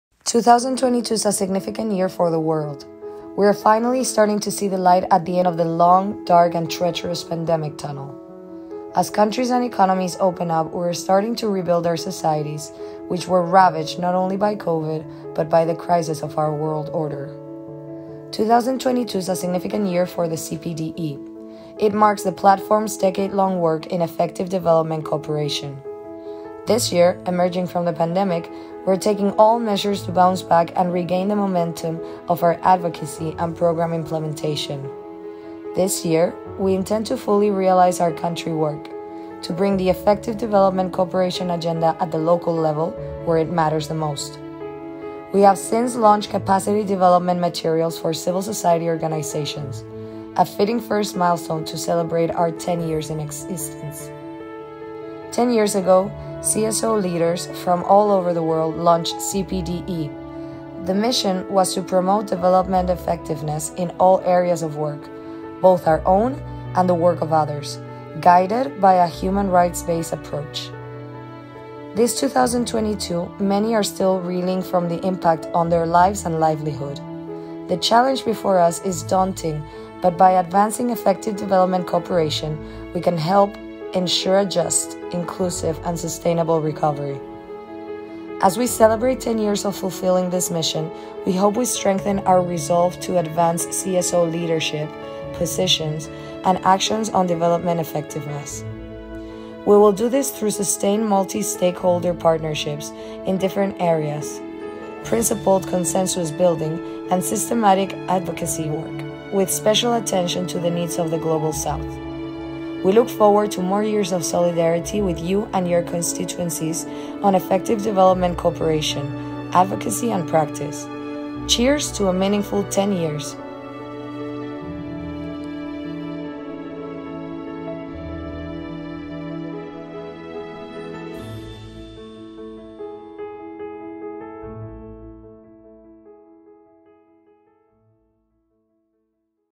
CPDE @ 10 - A decade of advancing EDC advocacy and practice
2022 is a significant year for the world. We are finally starting to see the light at the end of the long, dark and treacherous pandemic tunnel. As countries and economies open up, we are starting to rebuild our societies, which were ravaged not only by Covid, but by the crises of our world order.
2022 is also a significant year for CPDE.
It marks the platform’s decade-long work in effective development cooperation.